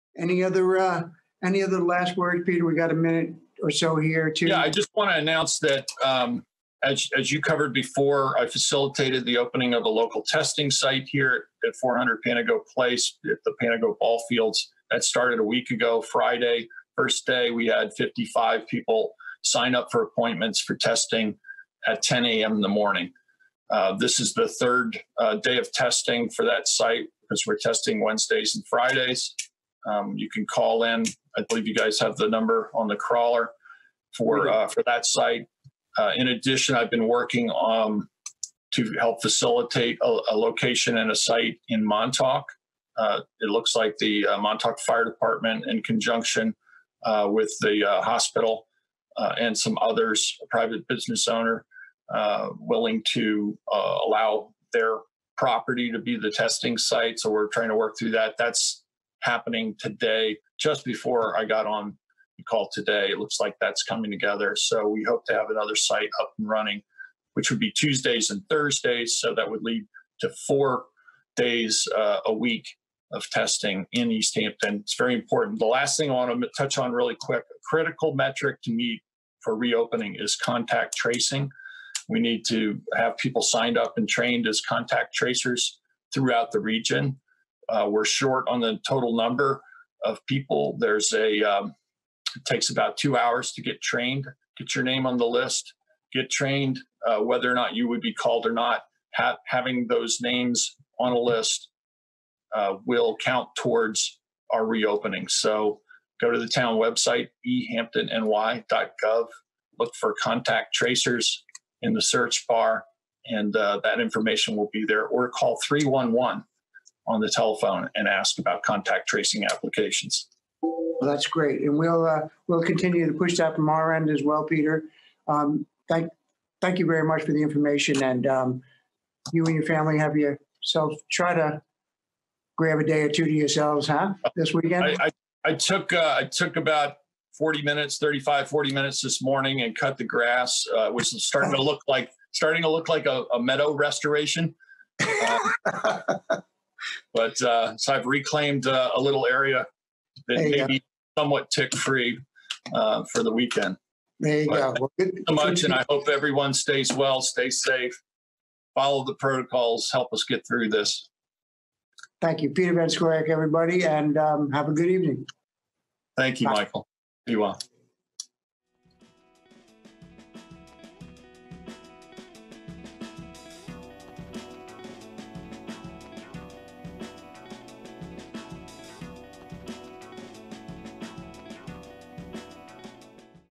Facts@Five - Peter Van Scoyoc announces new COVID-19 testing site in Montauk - May 22, 2020
In an interview with Michael Clark, Peter Van Scoyoc announces a new COVID-19 testing site in Montauk - May 22, 2020